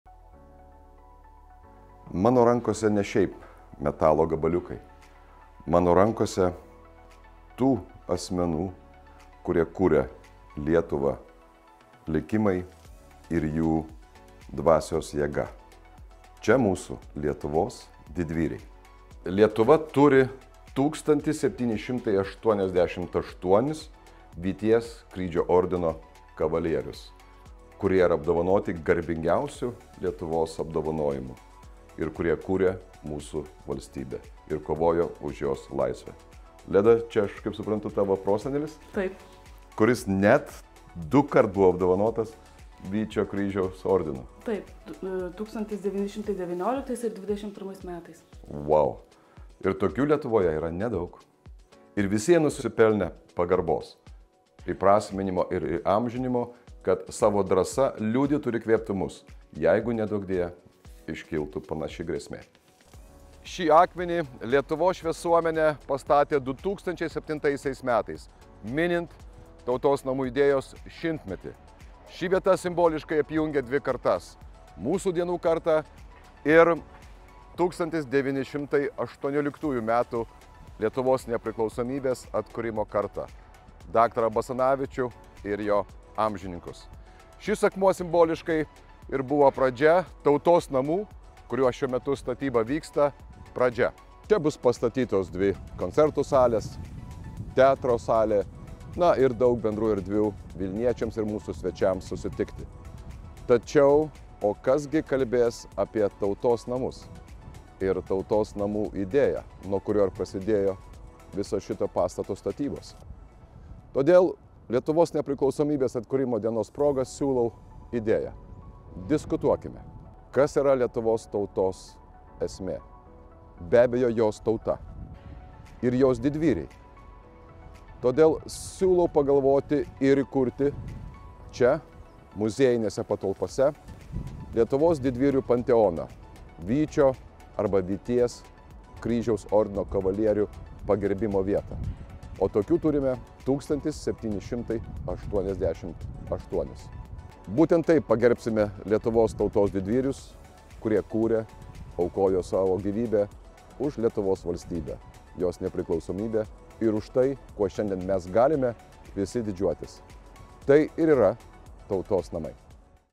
Tautos namai Vilniuje!
Vieni tai vadina koncertų rūmais, kiti Tautos namais- taip, kaip tai
pirmasis prieš daugiau nei šimtą metų pavadino daktaras Jonas
Basanavičius. Dėl koncertų salės kaip ir viskas aišku. Tačiau Tautos
namams reikia turinio… ar idėjos. Vieną siūlau ir kviečiu diskusijai.
Lietuvos valstybė už atsidavimą ir pasiaukojimą savo sūnus ir dukras
pagerbia Vyčio Kryžiaus ordinu. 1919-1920 metais kovose už Lietuvos
laisvę ir valstybingumą dalyvavo apie 30 tūkstančių kovotojų. 1788 iš jų
atliko išskirtinius asmeninius žygdarbius ir buvo apdovanoti Kryžiumi
,,Už Tėvynę’’- už narsą skirtu Vyčio Kryžiumi.
Statydami koncertų salę, turime pareigą prisiminti ir Tautos namų idėją,
kuri įamžintų Lietuvos kovotojus už laisvę.
Todėl vienoje iš salių siūlau įrengti Vyčio Kryžiaus ordino - didvyrių
panteoną.
Švęskime Lietuvą – darbu jai. Su Vasario 16-ja!